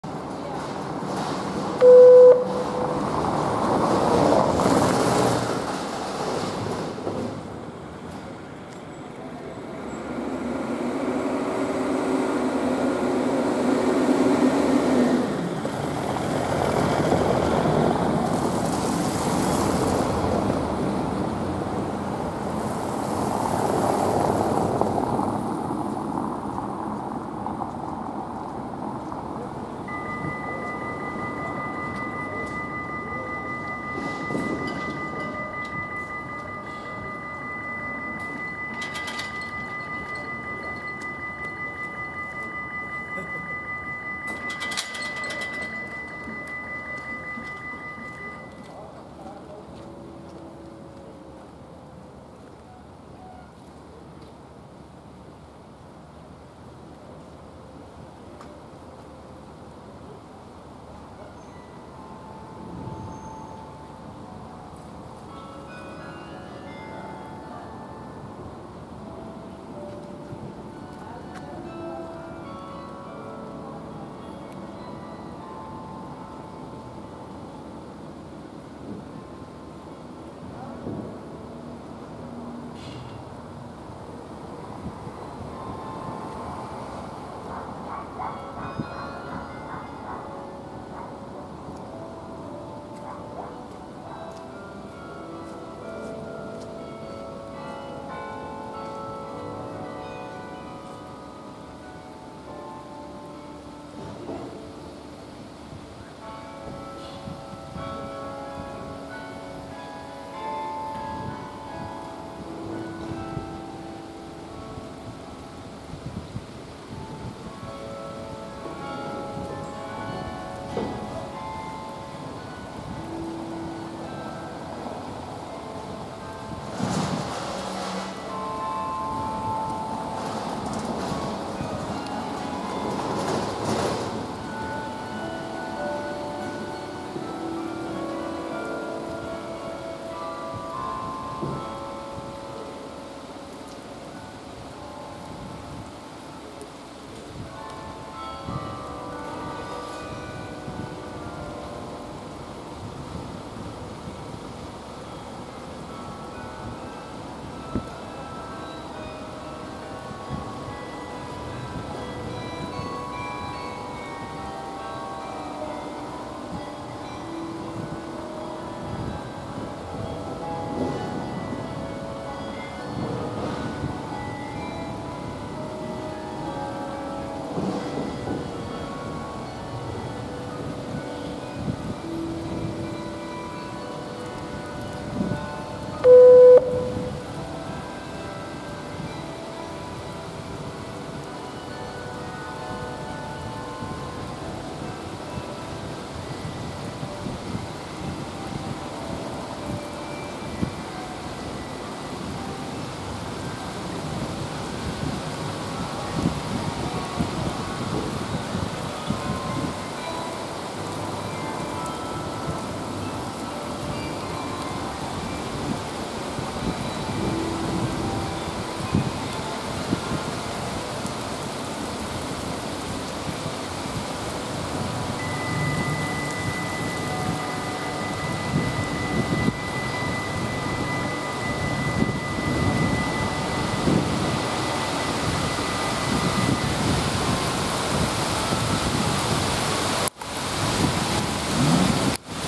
Brugopening Engelenburgerbrug Dordrecht Basculebrug/ Basculebridge/ Pont Basculant/ Klappbrücke
De Engelenburgerbrug in de Nieuwe Haven gaat open voor een boot

Le Pont de Engelenburger ouvert pour un navire

The Engelenburgerbridge opens for a vessel

Die Engelenburgerbrücke öffnet für ein Boot

d.d. 26 augustus 2023